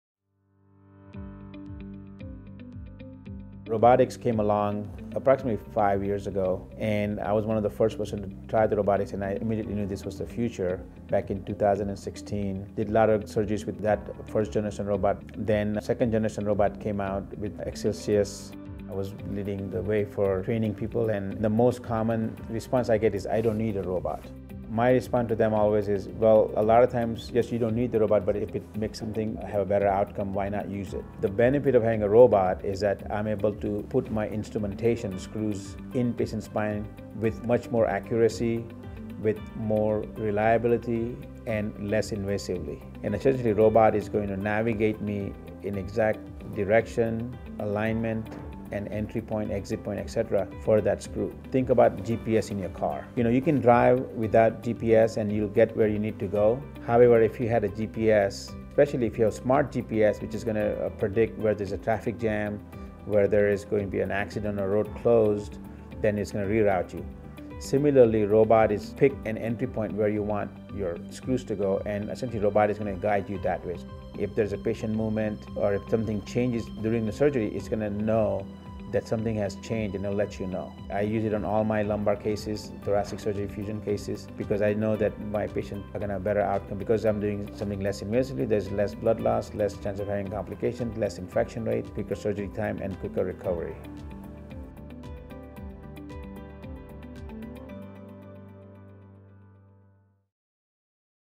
Fort Worth Brain and Spine Institute - Dr. Samir Lapsiwala - Robotic Assisted Spine Surgery
A form of minimally invasive spine surgery, Robotic spine surgery utilizes a robotics system to assist and perfect surgical robotics spinal surgery intervention for patients experiencing chronic back pain unresponsive to non-surgical treatment.  Designed to improve accuracy and optimize patient care, much like a GPS in your car. Many surgeons report several key surgical benefits for patients included but not limited to shorter hospital stays, less tissue damage, and smaller incisions (which may lead to smaller scars).